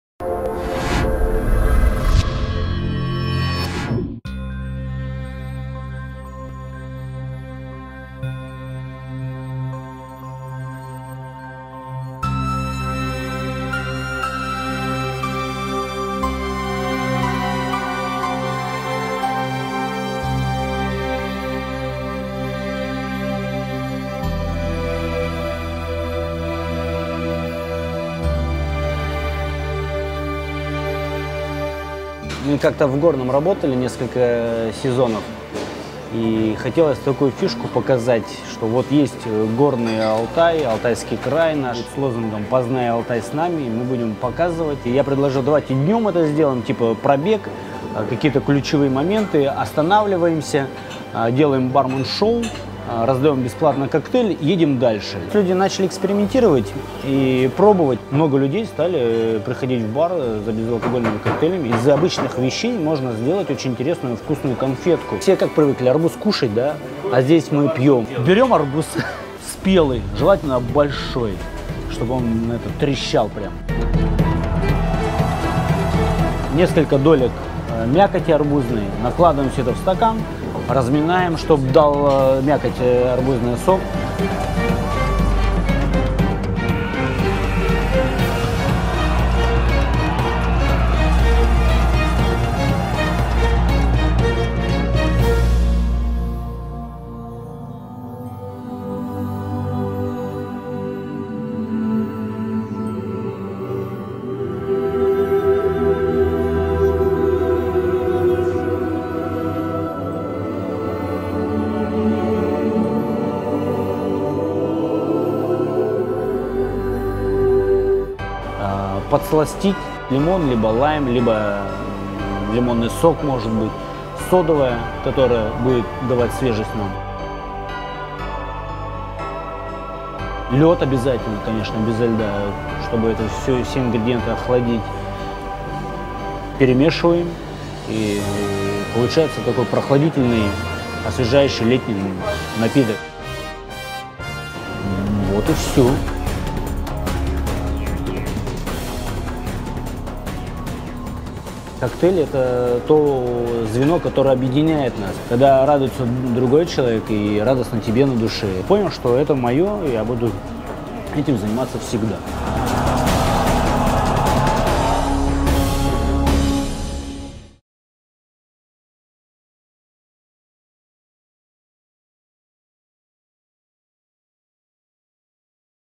Андрей Сапсаев о коктейль турне 2013
ВСТРЕЧА С СОЛНЦЕМ. (безалкогольный коктейль)
свежий Арбуз - 150гр
лайм - 40гр
сироп дикая слива - 30мл
содовая - 200мл
украшен: мята цедра апельсина

Коктейль-турне 2013
Это безалкогольное мероприятие проводится для пропаганды здорового образа жизни, повышения профессионального уровня барменского искусства и привлечения туристов в Алтайский край.
Видеооператоры и фотографы студии SINTES.TV зафиксируют происходящее и периодически, после прохождения стоянок, будут размещать фотографии видео, на портале SINTES.TV, где будет организован профильный сайт в поддержку этого турне.